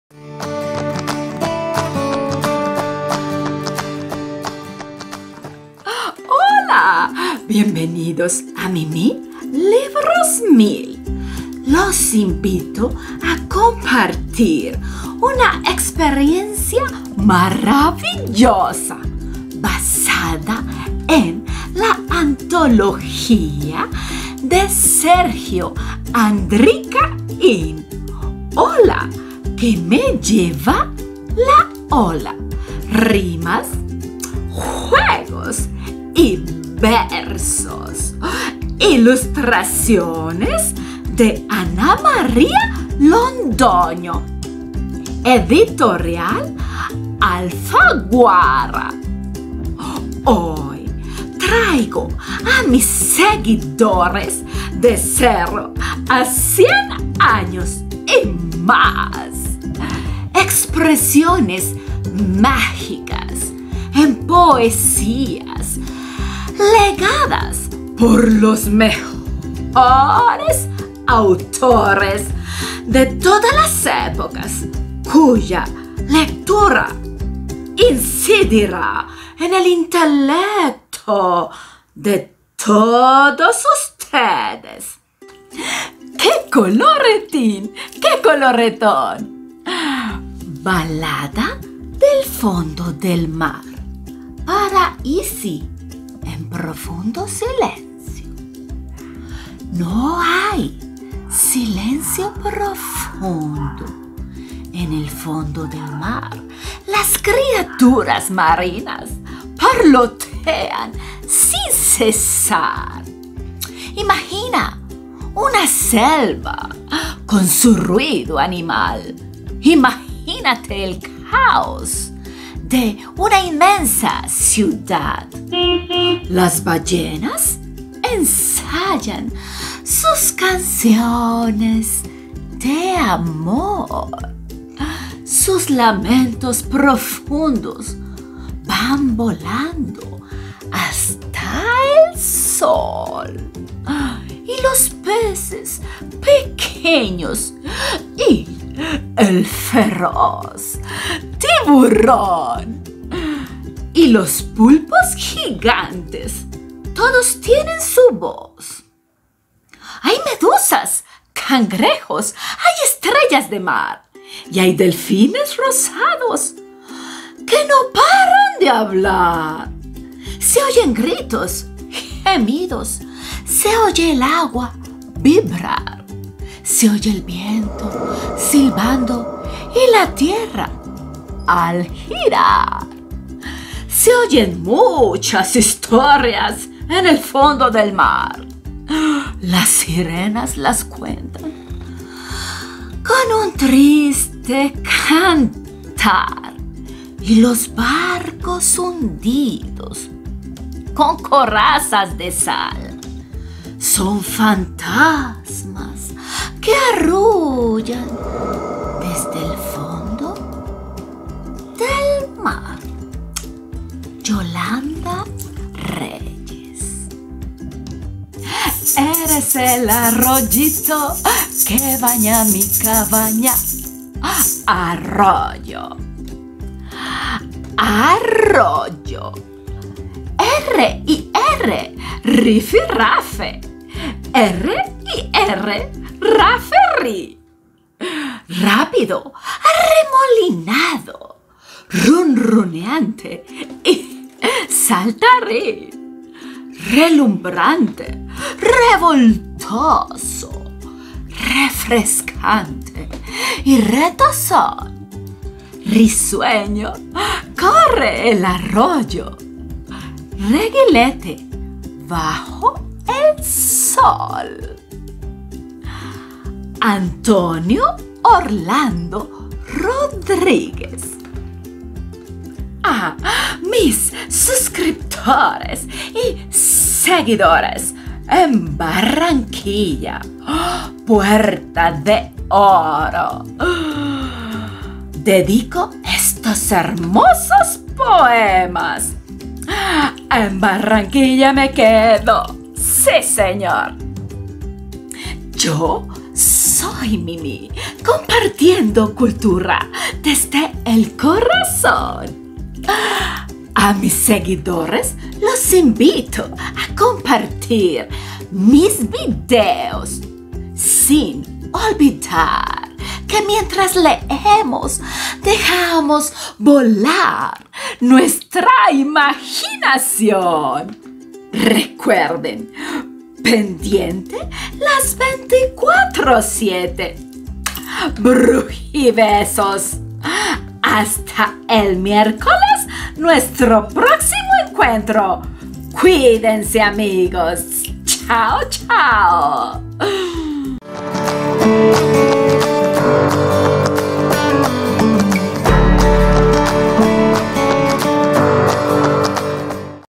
18.🧜‍♀️ POEMAS CORTOS PARA NIÑOS👋¡HOLA!que me lleva la ola🌊YOLANDA REYES🐚MIMI LIBROS MIL🧜‍♂️🌊
"Esta antología, elaborada por Sergio Andricaín, pone al alcance de la familia y de todos aquellos que leen con los niños, la mejor poesía de todas las épocas."